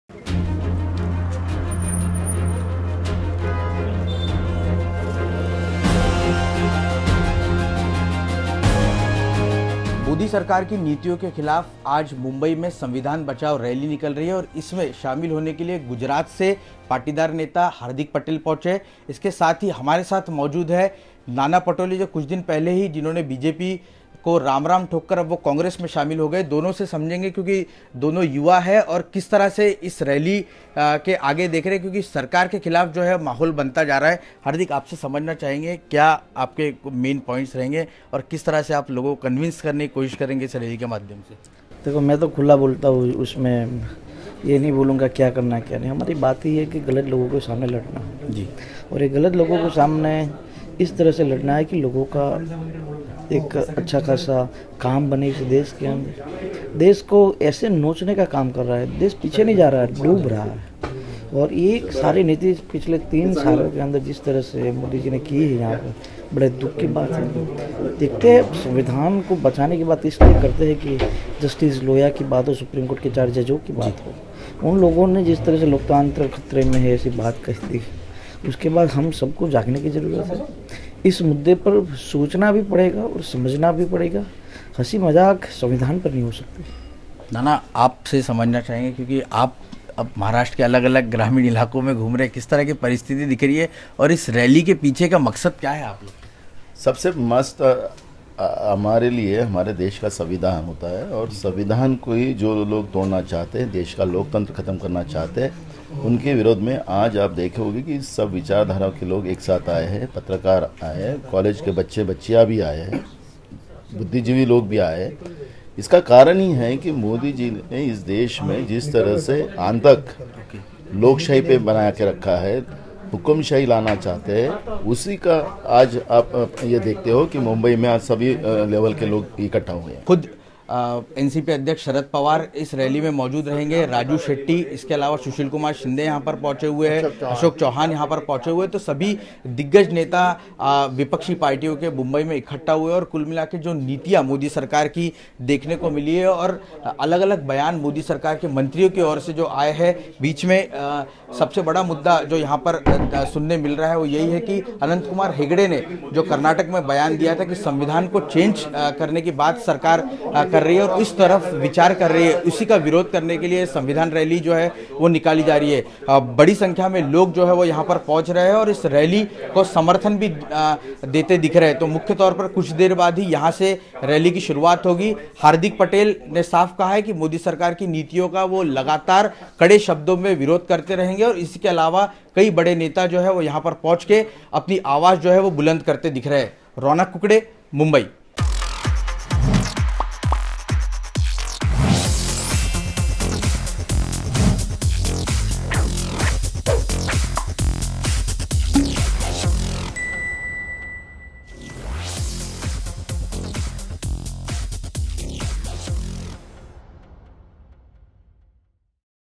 मुंबई में बोले हार्दिक: सरकार की नीतियों की वजह से डूब रहा है देश- Quint Hindi
मुंबई में 'संविधान बचाओ रैली' से पहले यहां विपक्षी नेताओं शरद पवार, शरद यादव, डी राजा, हार्दिक पटेल, उमर अब्दुल्ला, दिनेश त्रिवेदी और सुशील कुमार शिंदे ने आज बैठक की. यह मुलाकात महाराष्ट्र विधानसभा में विपक्ष के नेता विखे पाटिल के आवास पर हुई. राकांपा के नेता प्रफुल्ल पटेल और डीपी त्रिपाठी तथा पूर्व सांसद राम जेठमलानी भी इस बैठक में मौजूद थे. तृणमूल कांग्रेस ने इस मार्च में हिस्सा लेने के लिए पूर्व रेल मंत्री दिनेश त्रिवेदी को भेजा है.